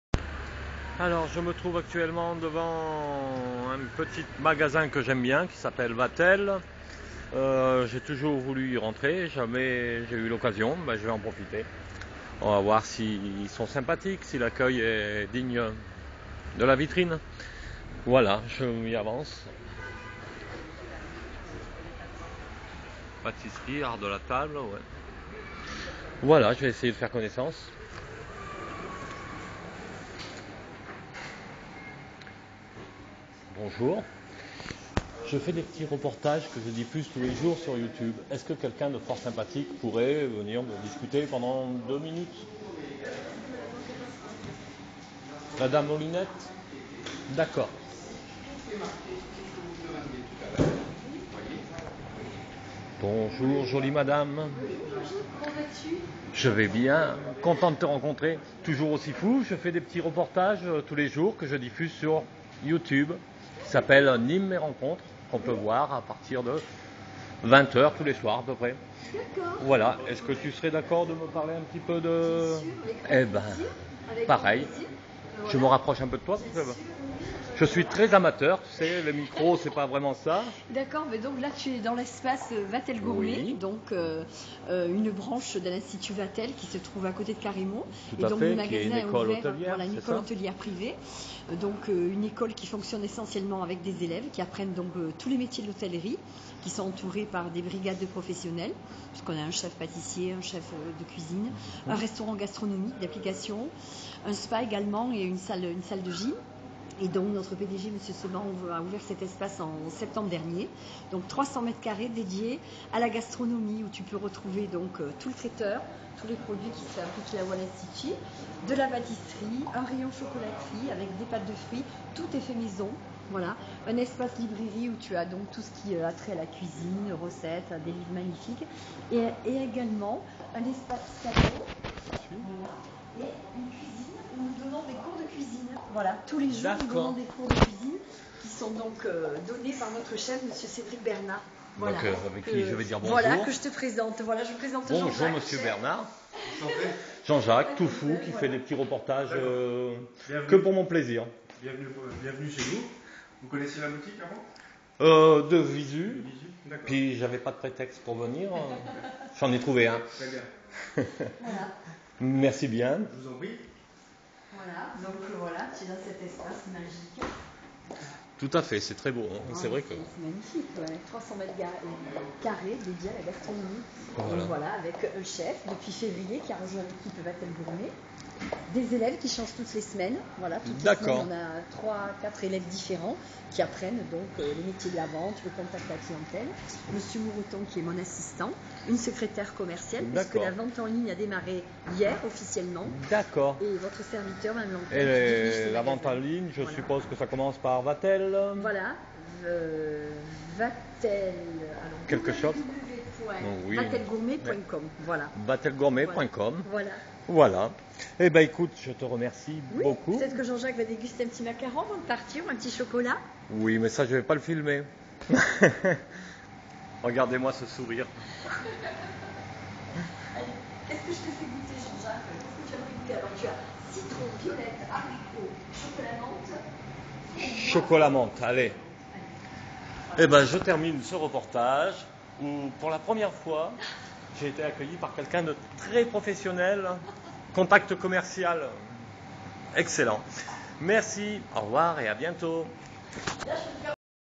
Nîmes Mes Rencontres // 0013
École Hôtelière Vatel l'espace gourmet présenté par la jolie Myriam une adresse a conseiller absolument a deux pas des Halles de Nîmes.